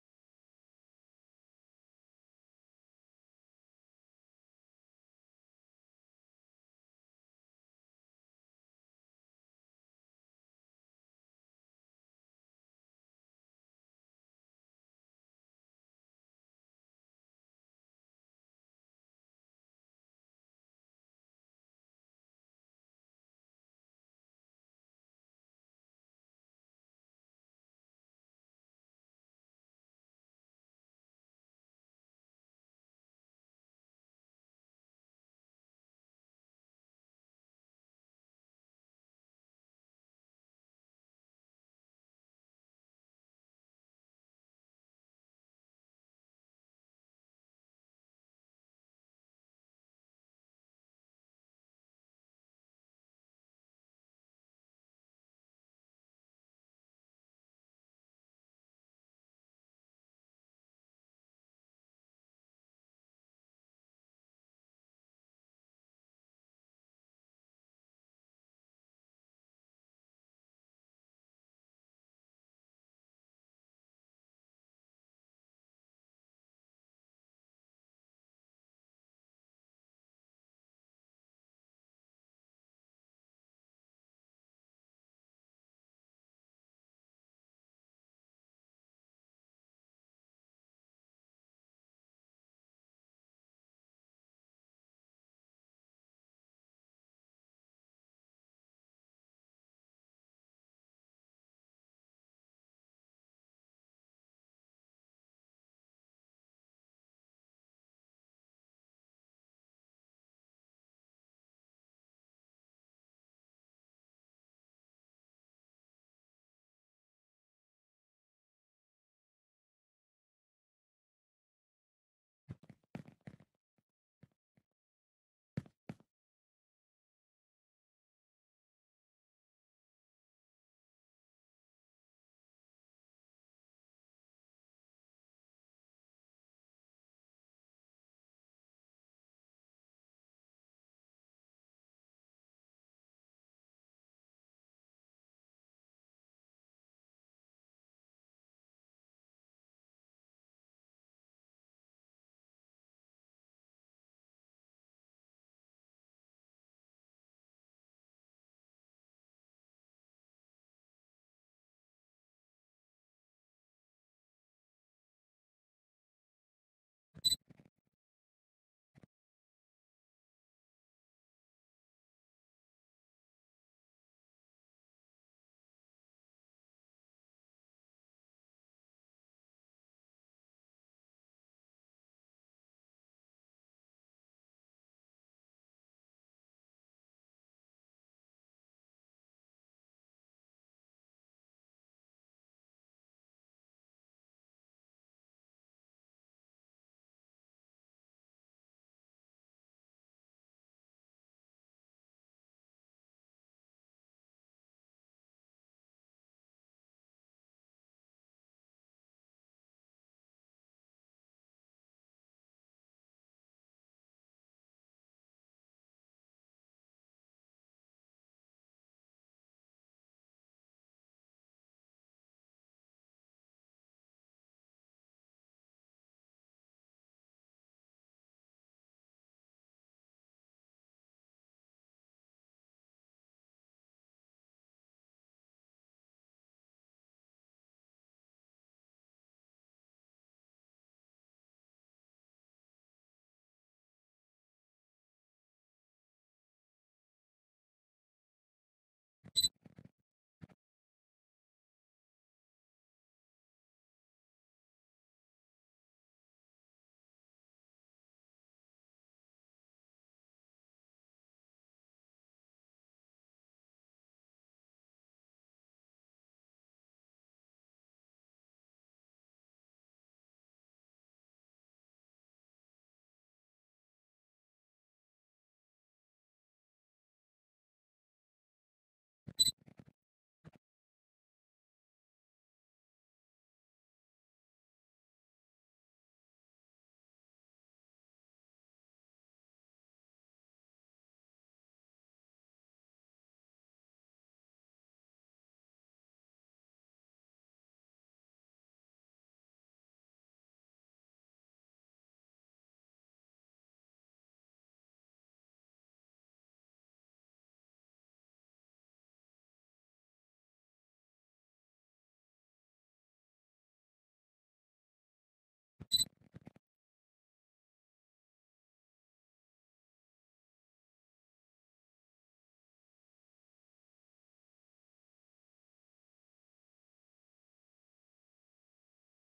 Level Design | Hong Kong Night Club Map for BTD | Unreal engine 5 | Lumen | Nanite | PART 2
The first stages of designing a night club map for 'Better Than Dead' using Unreal Engine 5.
This early version showcases the initial raw layout, lighting, and general shaping...